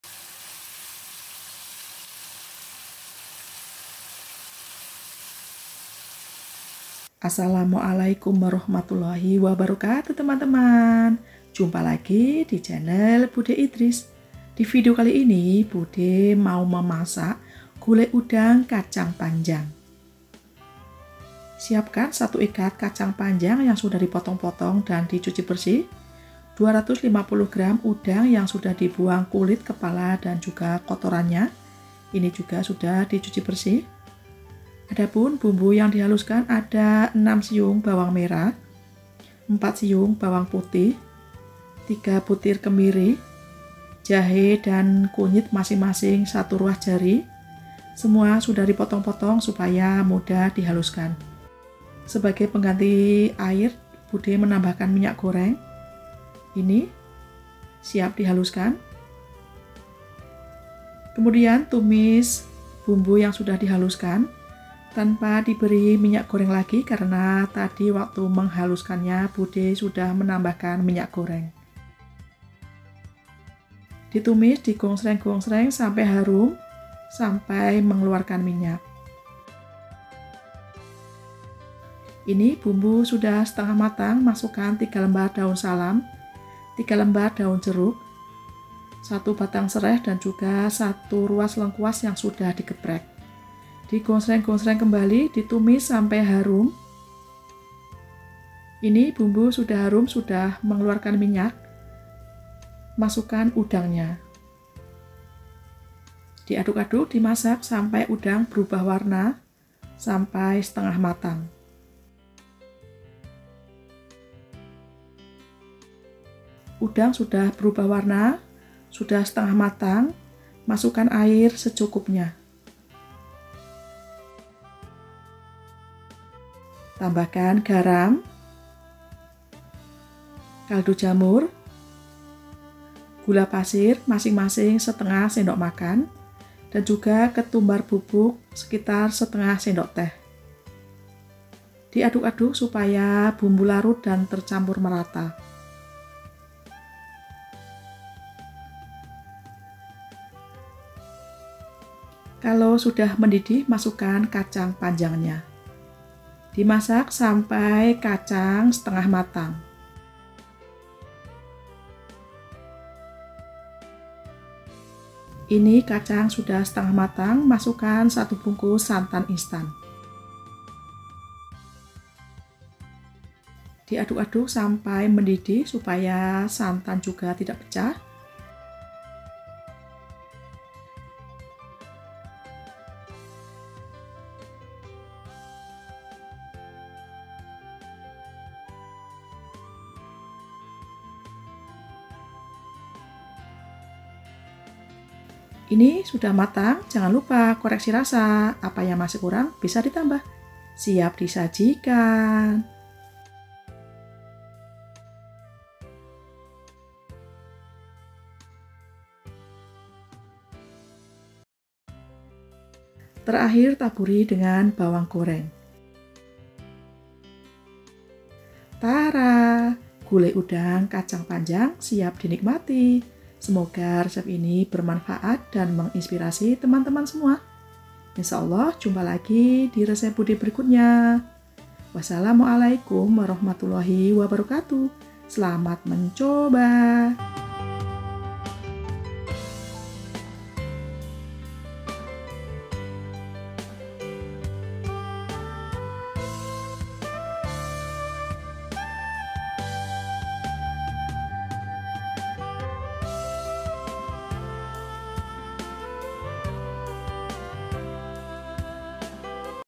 SEENAK INI ‼️ RESEP GULAI UDANG KACANG PANJANG
ASSALAMU'ALAYKUM WR ,WB.

  Hai semua divideo kali ini @Budheidries  memasak " GULAI UDANG KACANG PANJANG "

- 1 ikat kacang panjang
- 250 gram udang
- 6 siung bawang merah
- 4 siung bawang putih
- 3 butir kemiri
- 1 ruas kunyit
- 1 ruas jahe
- 3 lembar daun salsm
- 3 lembar daun jeruk
- 1 batang sereh
- 1 ruas lengkuas
- 1/2 sdm garam
- 1/2 sdm kakdu jamur
- 1/2 sdm gula pasir
- 1/2 sdt ketumbar bubuk
- 1 bungkus santan instan
bawang goreng,air dan minyak goreng.

WASSALAMU'ALAYKUM WR,WB.